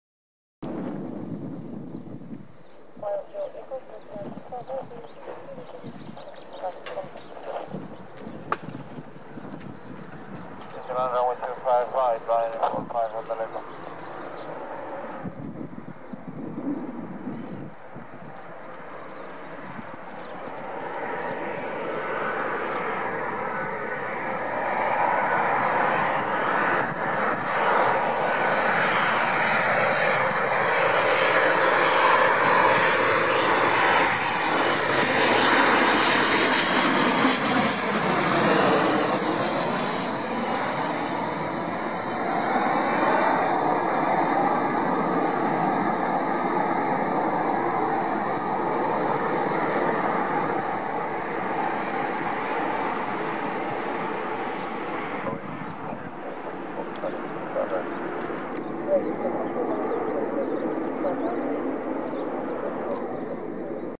Boeing 737-800 EI-EMA Ryanair landing at Barcelona-El Prat Airport (LEBL)
Seen on 25/04/2015.